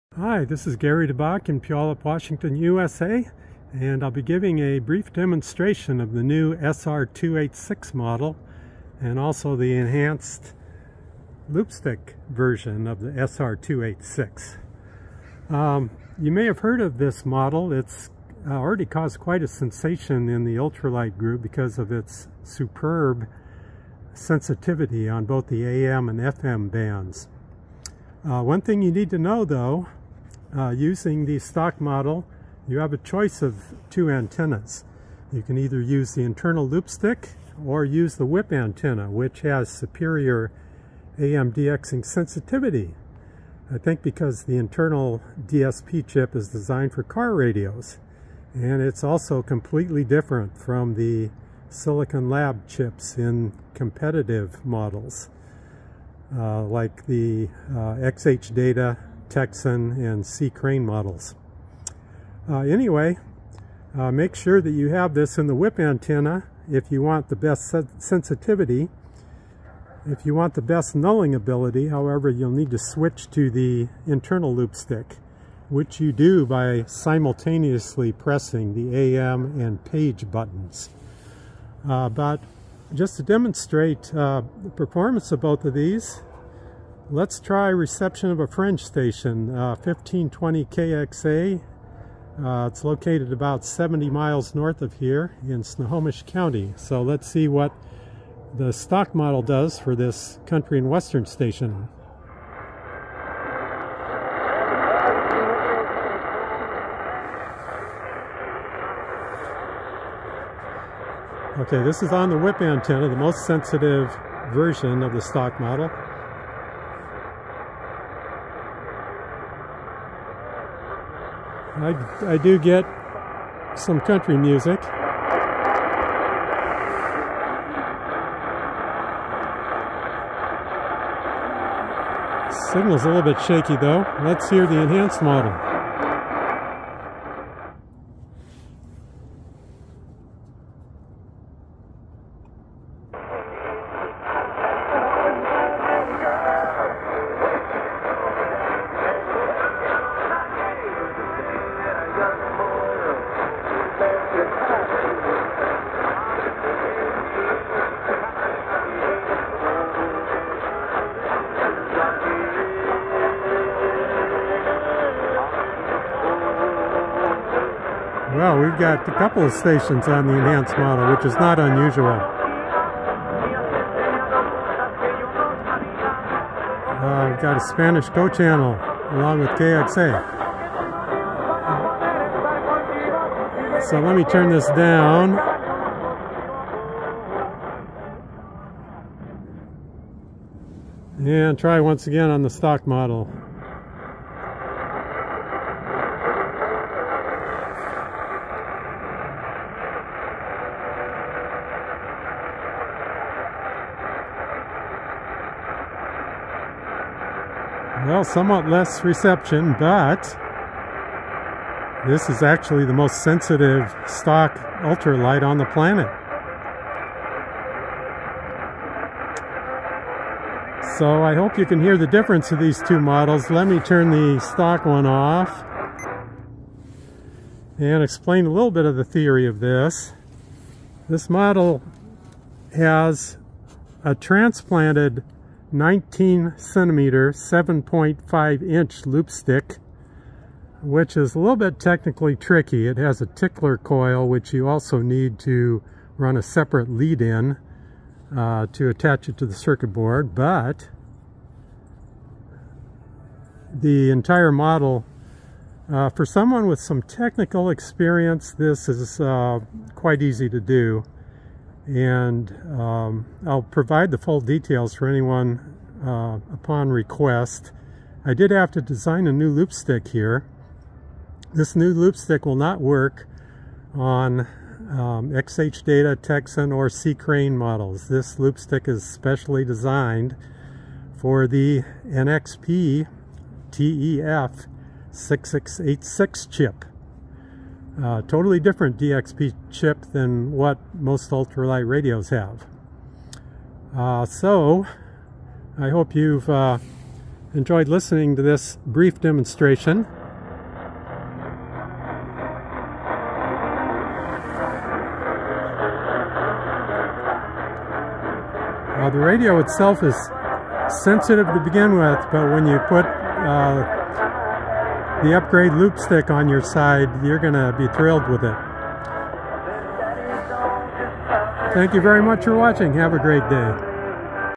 7.5 inch (19cm) AM Loopstick Qodosen SR-286 Ultralight Radio
Demonstration of the AM-DXing reception of a stock Qodosen SR-286 model, followed by the far stronger reception of the 7.5 inch loopstick model while receiving fringe station 1520-KXA in Snohomish, WA. The enhanced model uses a transplanted loopstick with a 638 uH main coil and a 4 uH tickler coil (93 turns of 175/46 Litz wire, and 2 turns of SAE 22 solid wire). These duplicate the inductances of the internal stock loopstick.